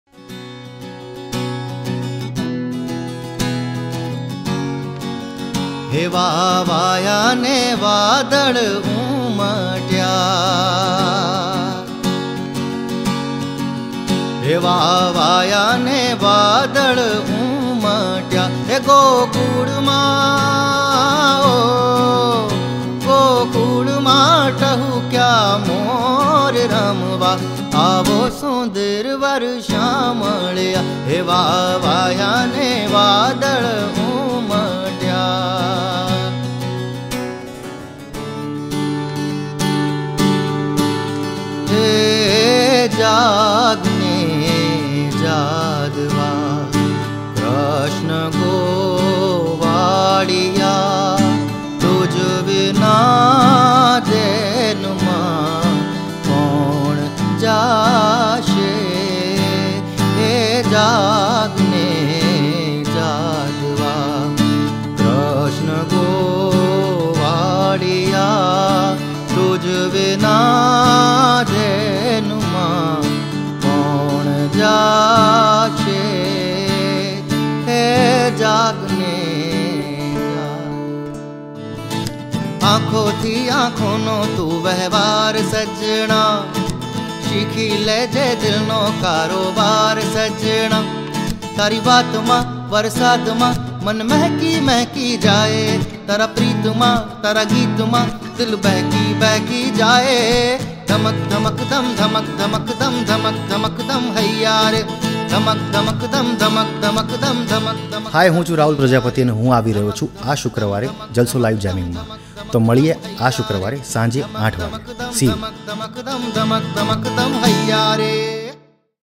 Rahul Prajpati | Live Jamming 3 | Teaser
જલસો Friday live Jamming માં આ શુક્રવારે અમારી સાથે જોડાશે જાણીતા ગિટાર પ્લેયર અને ગાયક રાહુલ પ્રજાપતિ